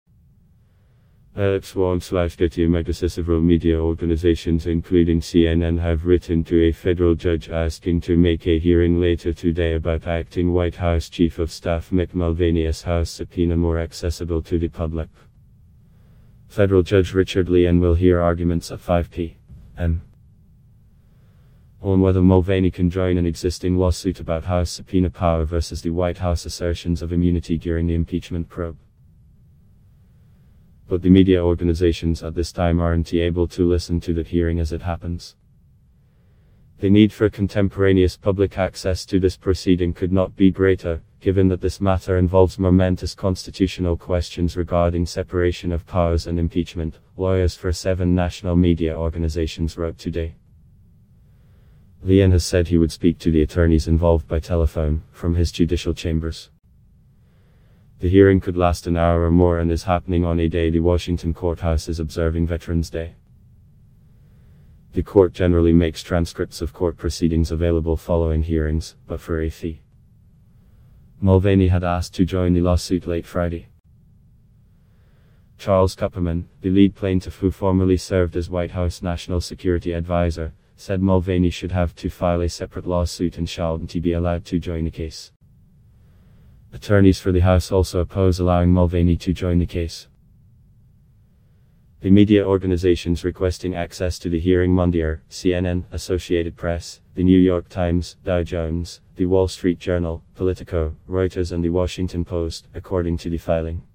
Media asks judge to make today's Mulvaney hearing public
Alex Wong/Getty ImagesSeveral media organizations including CNN have written to a federal judge asking to make a hearing later today about acting White House chief of staff Mick Mulvaney’s House subpoena more accessible to the public.Federal Judge Richard Leon will hear arguments at 5 p.m. on whether Mulvaney can join an existing lawsuit about House subpoena power versus the White House assertions of immunity during the impeachment probe.But the media organizations at this time aren’t able to listen to that hearing as it happens.“The need for contemporaneous public access to this proceeding could not be greater, given that this matter involves momentous constitutional questions regarding separation of powers and impeachment,” lawyers for seven national media organizations wrote today.Leon has said he would speak to the attorneys involved by telephone, from his judicial chambers.